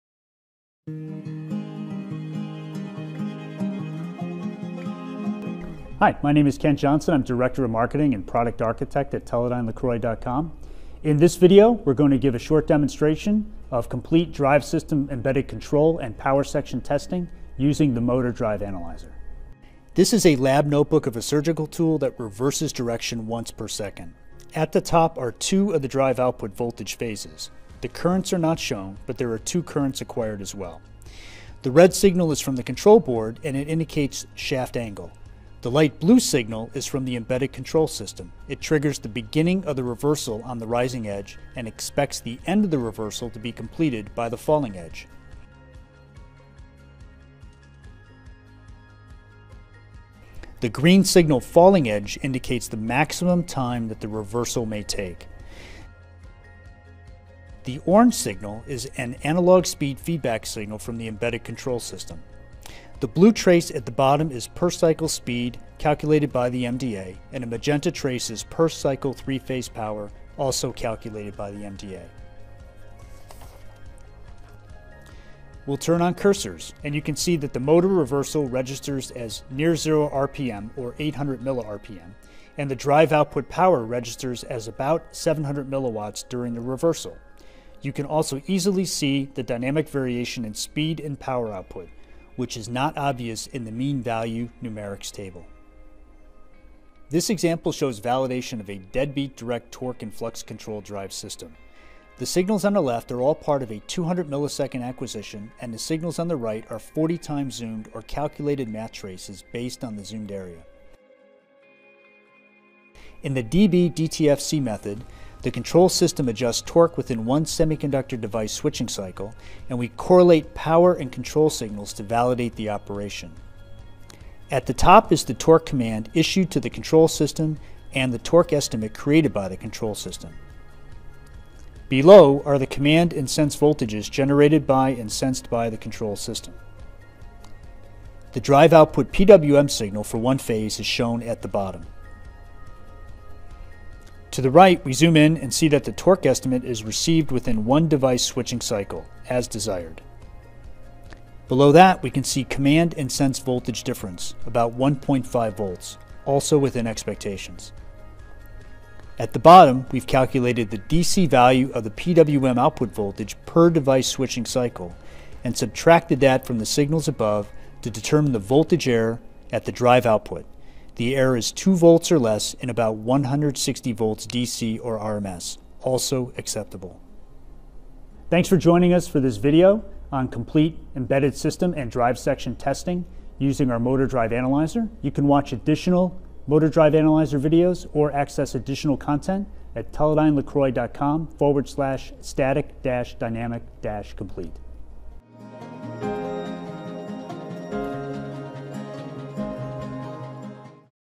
MDA 8000HD Complete Test Coverage
The Teledyne LeCroy MDA 8000HD tests complete embedded control systems and drive systems over times as short as a single device-switching cycle. It provides complete power section and embedded control test coverage.